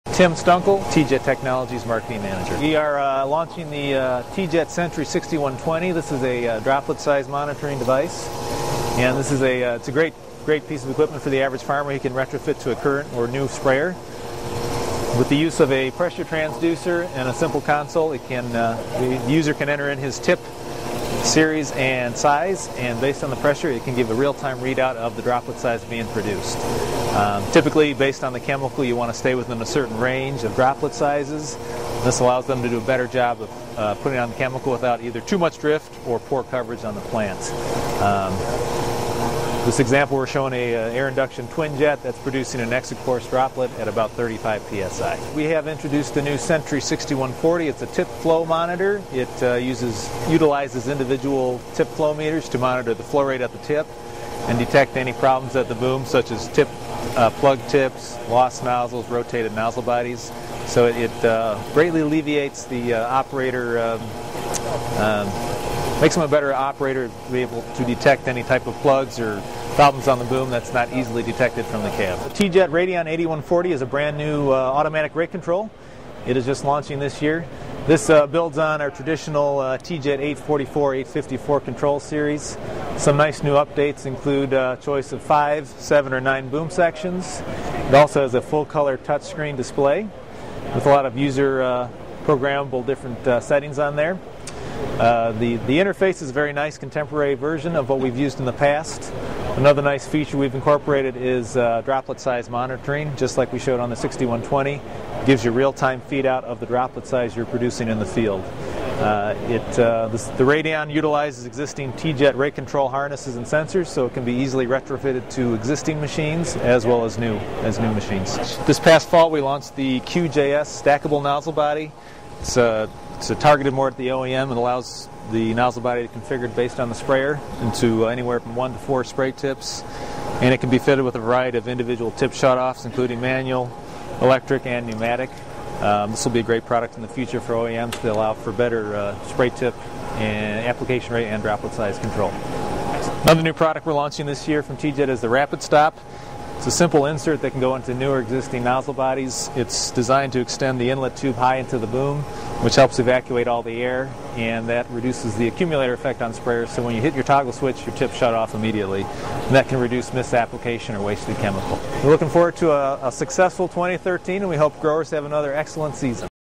TeeJet Introduces New Sprayer Components
Droplet-size and tip-flow monitors for sprayers are among the new products on display at TeeJet's 2013 Ag Connect booth. Marketing manager Tim Stuenkel provides a tour.